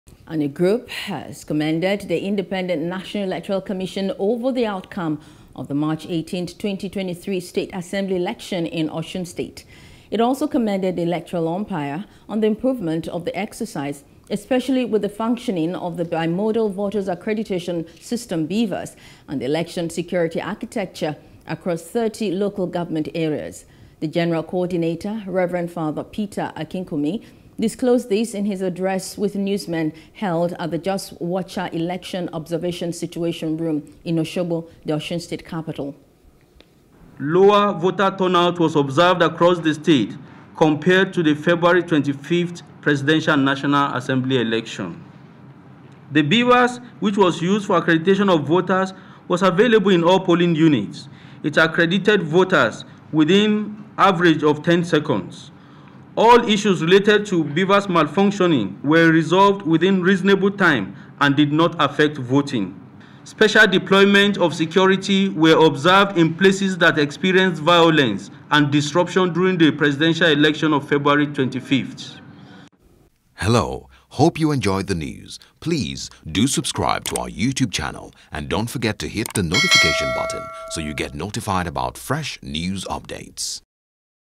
Assembly Election: Group Commends Electoral Process, Tasks INEC On Inclusivity Of Data For PWDs
A group has commended the Independent National Electoral Commission INEC over the outcome of March 18, 2023 state assembly election in osun state.